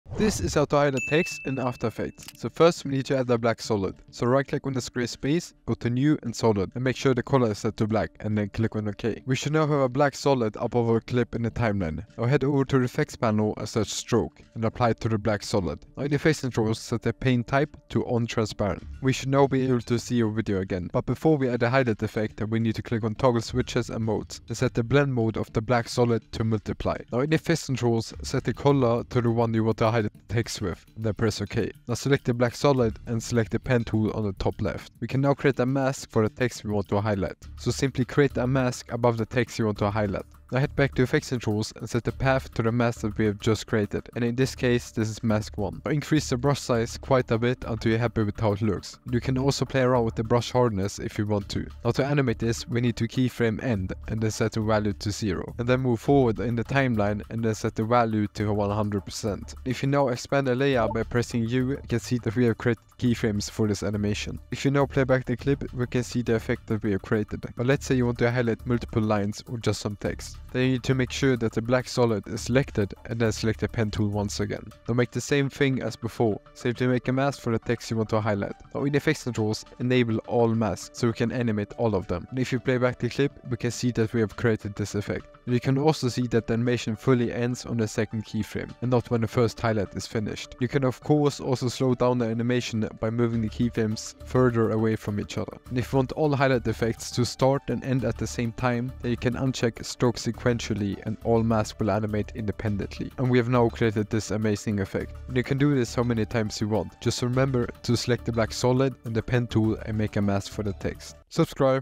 How To Highlight Text In After Effects
how to make highlight text animation in adobe after effects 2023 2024